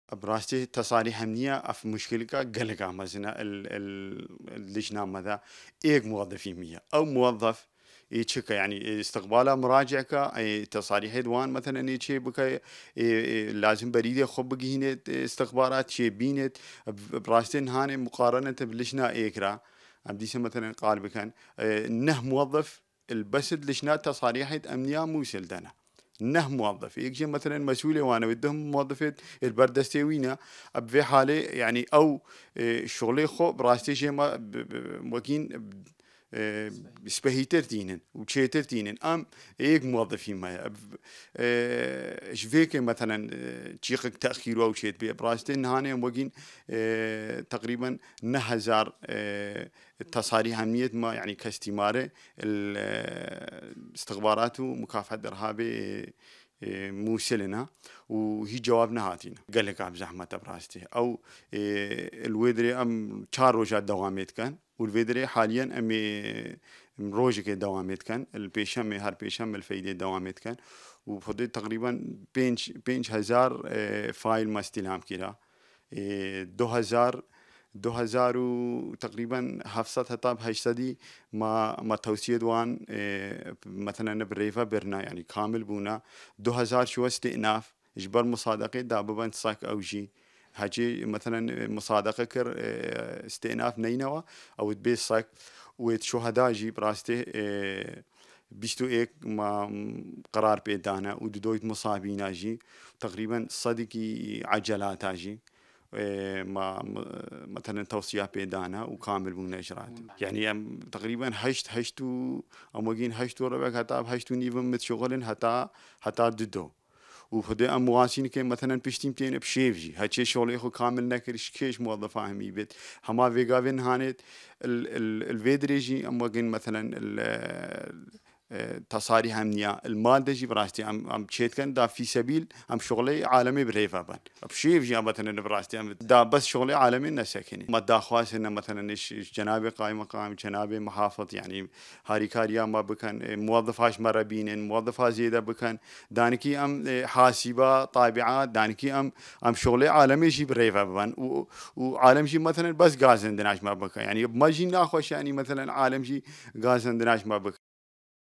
رێڤەبەرێ فەرمانگەها قەرەبووکرنا خەلک شنگالێ ساهر تاهر ژبوی سەما بەحسا کارێ لژنا قەرەبووکرنێ دکەت
رێڤەبەرێ فەرمانگەها قەرەبووکرنا خەلک شنگالێ ساهر تاهر ل لێدوانەکێدا ژبوی سەما تیڤی بەحسا کارێ لژنا قەرەبووکرنێ دکەت